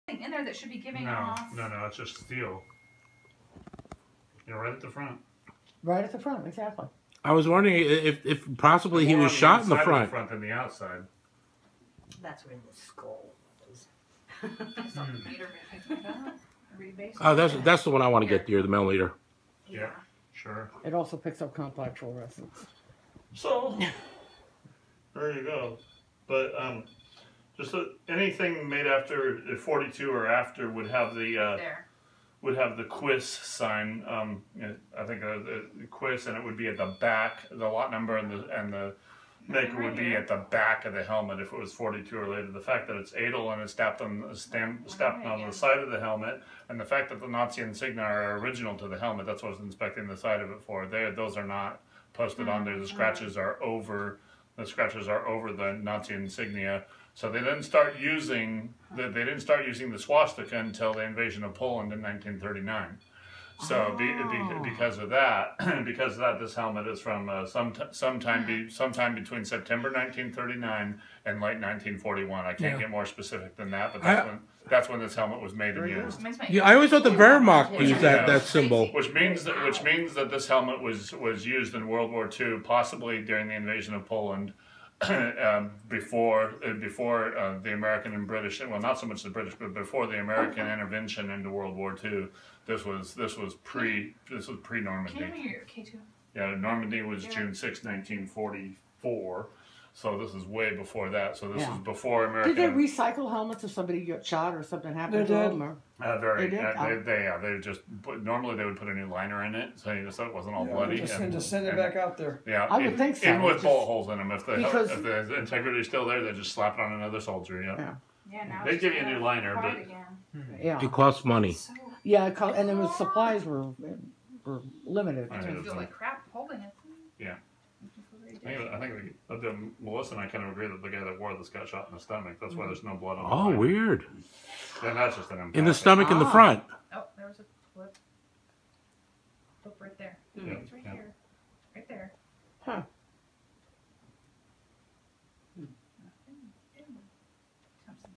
helmet part 1
SACSpirit investigating my Nazi police helmet part 1